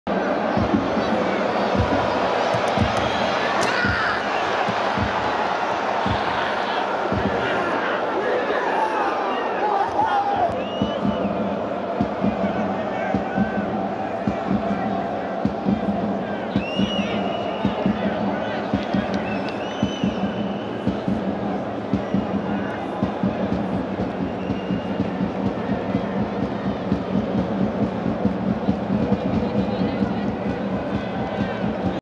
Ennin`s goal in the match against FC Rostov
The first goal by Richlord Ennin in RPL was beautiful
How many will he score by the end of the season?